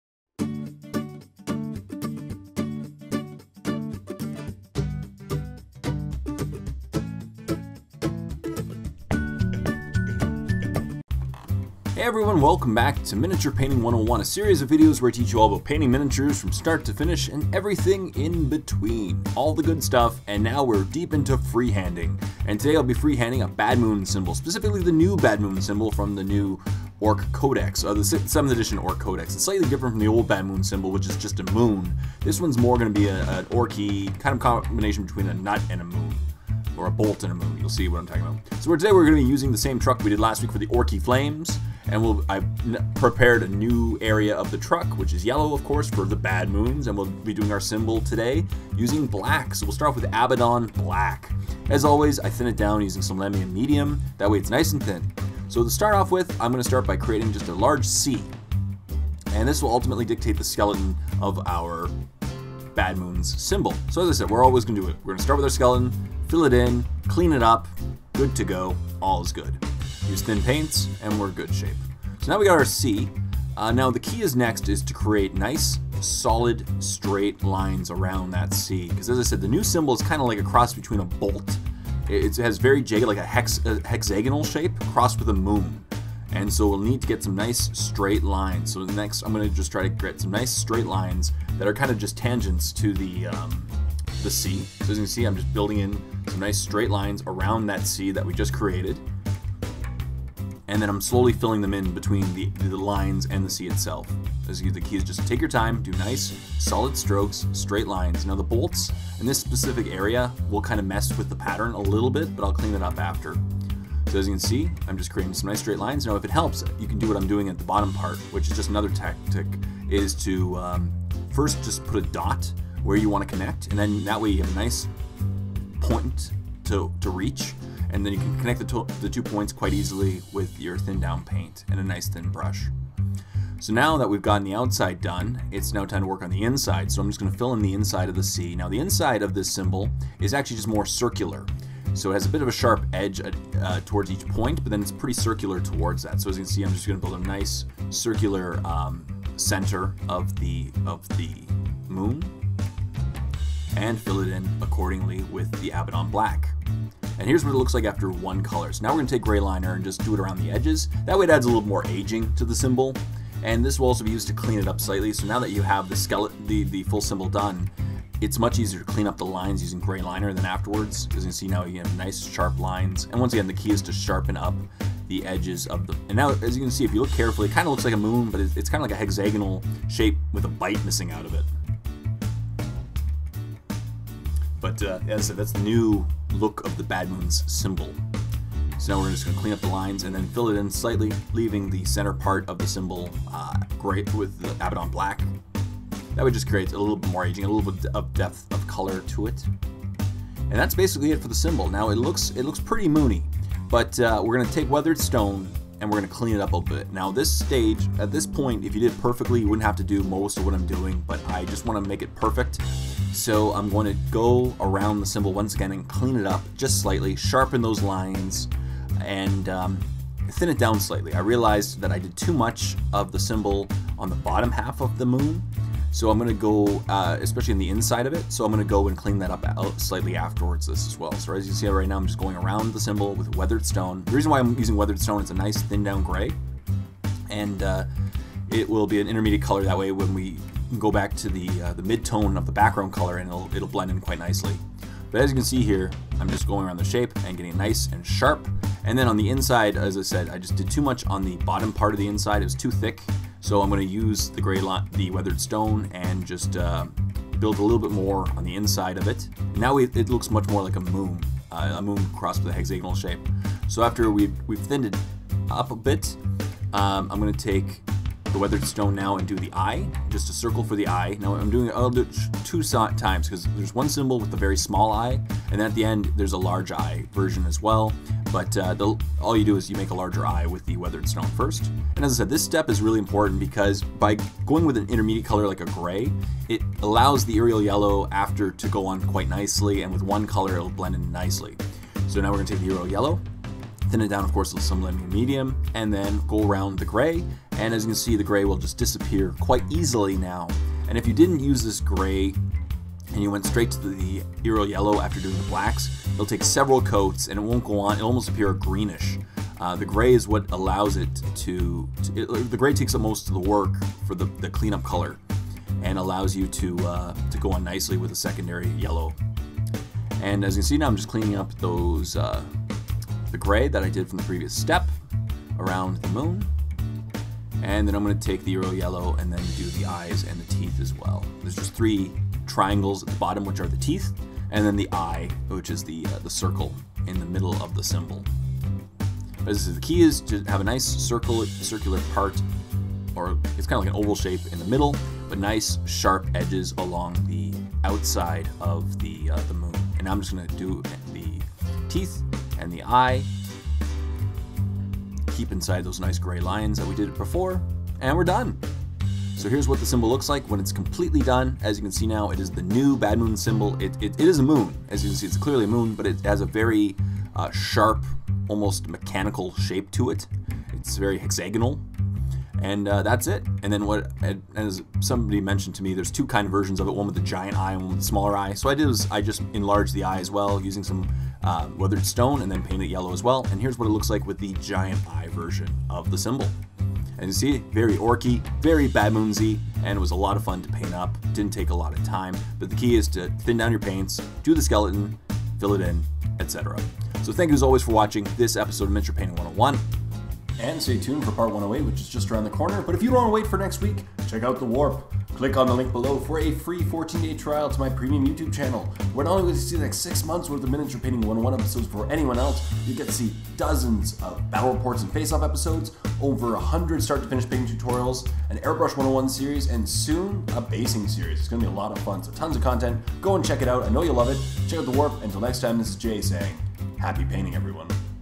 Miniature Painting 101: Part 107 - Freehanding the New Bad Moons Symbol
In this episode, Jay shows how to freehand the new(ish) Bad Moonz Ork Symbol on a Trukk. Enjoy.